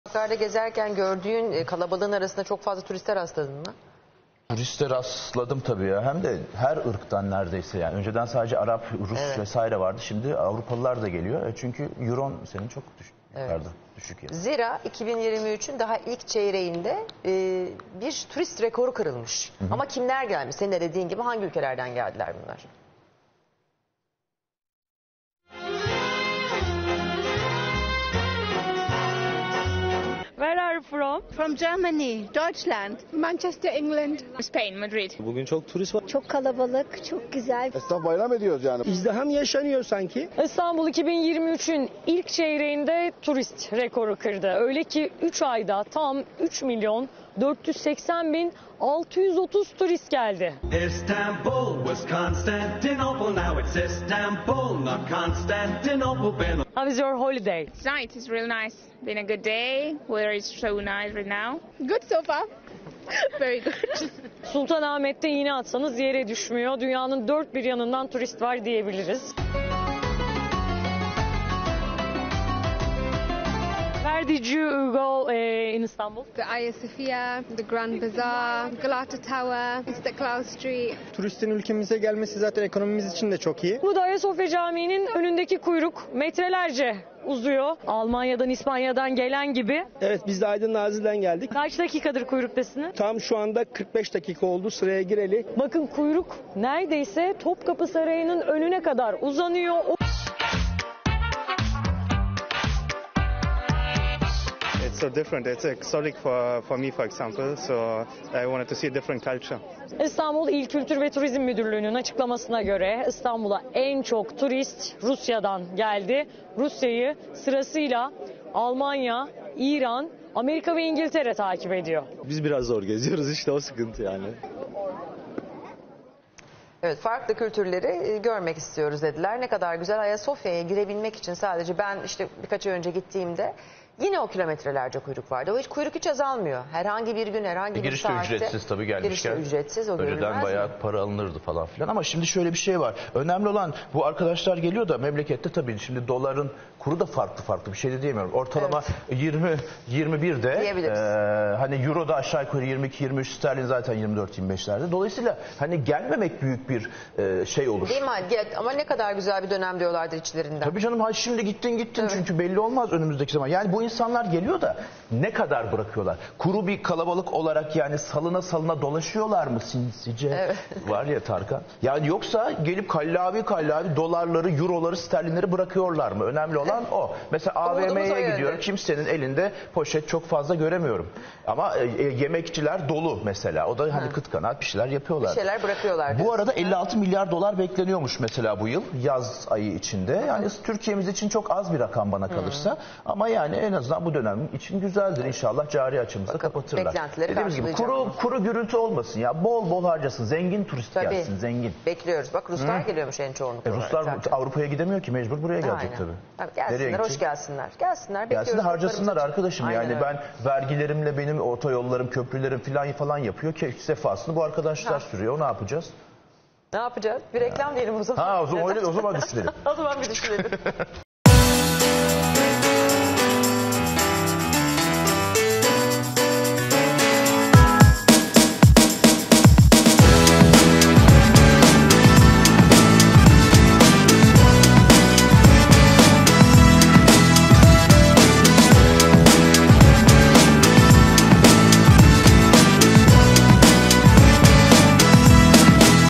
İstanbul, 2023 yılının ilk çeyreğinde turist rekoru kırdı!
İstanbul'a 2023'ün ilk üç ayında tam 3 milyon 480 bin 630 turist geldi. Turistler İstanbul'un tarihi yerlerini ve doğal güzelliklerini gezerken İstanbul'a en çok turist Rusya, Almanya, İran, ABD ve İngiltere'den geldi.